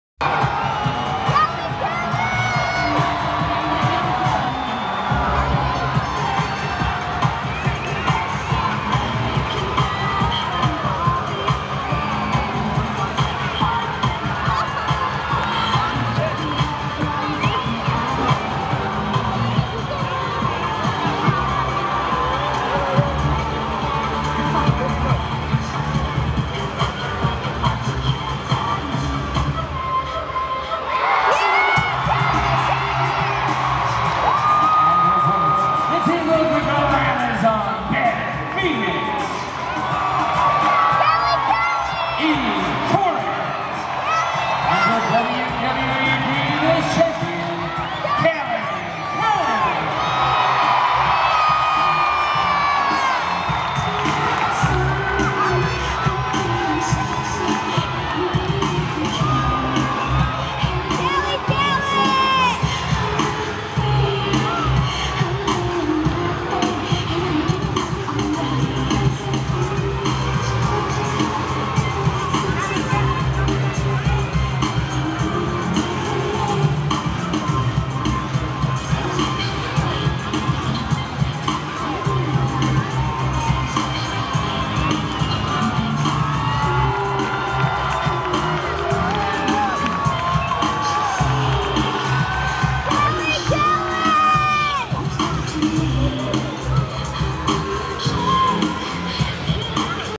6 Diva Tag Team Entrances
Maryse & The Bellas make their entrances ss do their opponents Eve Torres, "The Glamazon" Beth Phoenix, and The Divas Champion Kelly Kelly at the Raw World Tour in Houston on June 26th, 2011.